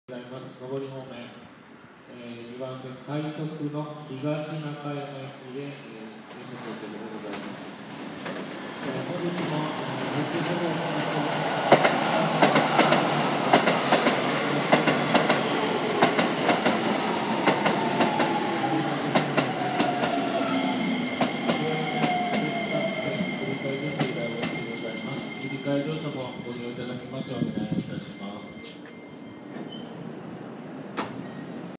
京成電鉄 快速東中山行き【脱線事故による行き先変更】
京成電鉄の快速東中山行きです。脱線事故の影響で行き先が変更されています。